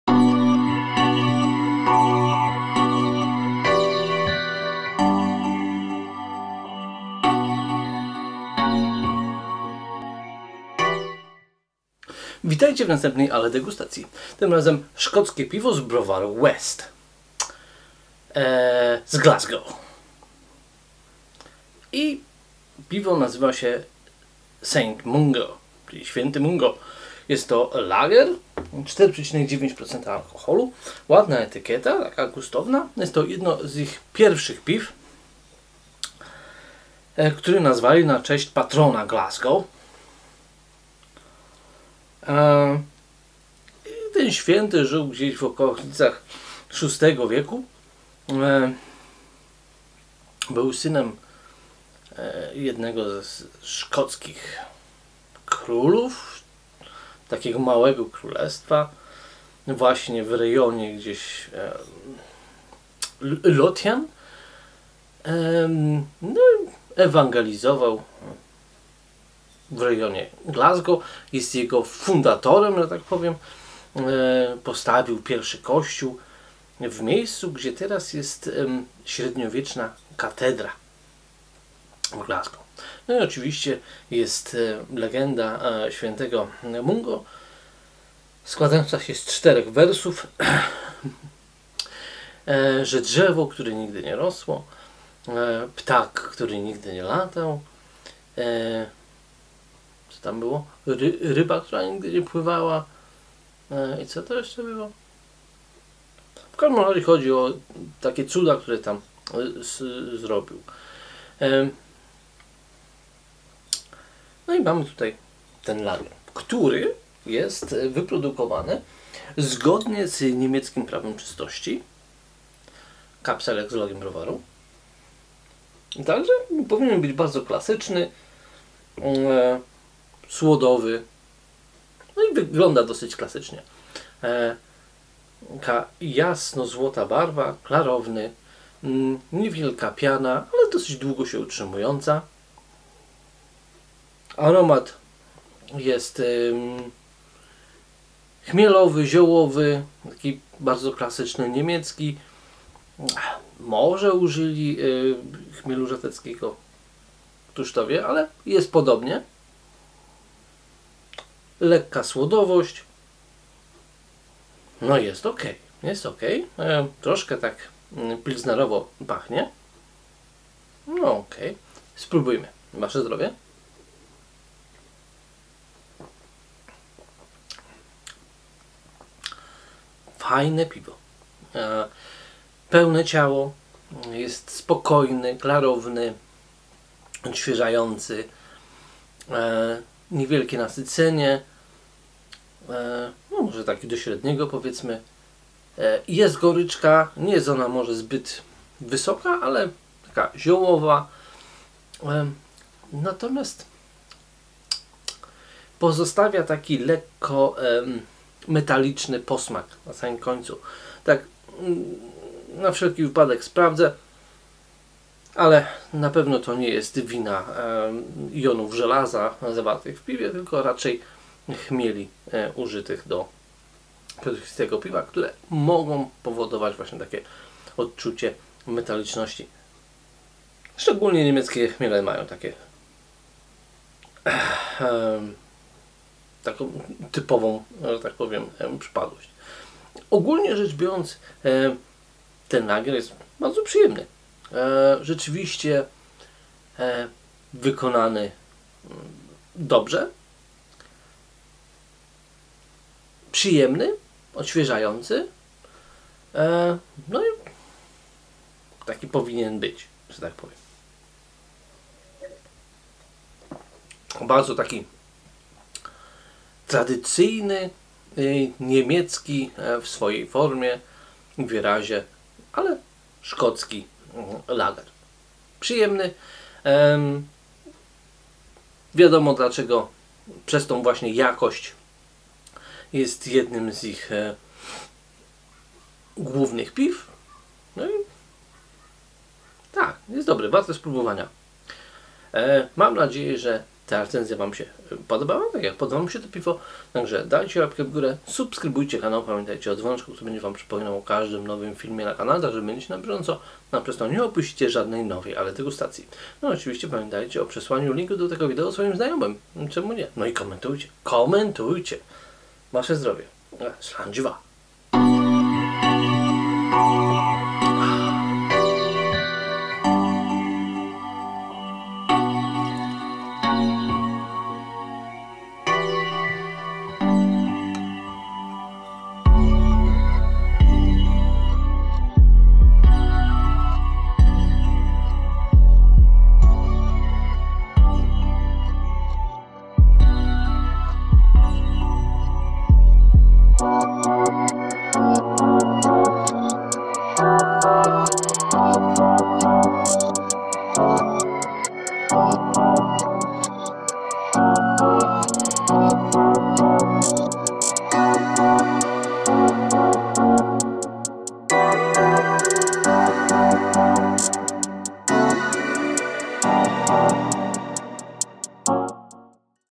West St Mungo PL
Ale Degustacja szkockiego lageru: West St Mungo

Warzone przez West Brewing Co.

Tu zaczęła się nasza historia. Pierwsze piwo uwarzone przez WEST musiało być hołdem zarówno dla naszego niemieckiego dziedzictwa, jak i dla naszego nowego rodzinnego miasta. Nazwany na cześć Świętego Patrona Glasgow, który był również zapalonym piwowarem, St Mungo jest doskonałym przykładem piwa Helles w stylu bawarskim. Odrobina jasnego słodu monachijskiego w zacierze nadaje piwu delikatny miedziany kolor i złożony smak słodowy. St Mungo to pięknie zbalansowane piwo z lekką ziołową nutą w tle i zakończeniem cytrusową, pikantną świeżością.